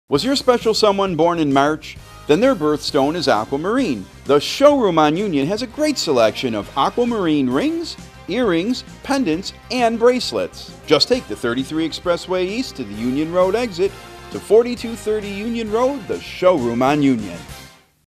Who Has March Birthstone Jewelry in Buffalo NY? The Showroom on Union Has Aquamarine Jewelry
Was your special someone born in March? Then their birthstone is aquamarine. The Showroom on Union has a great selection of aquamarine rings, earrings, pendants and bracelets. Just take the 33 expressway east to the Union Road exit to 4230 Union Road The Showroom on Union.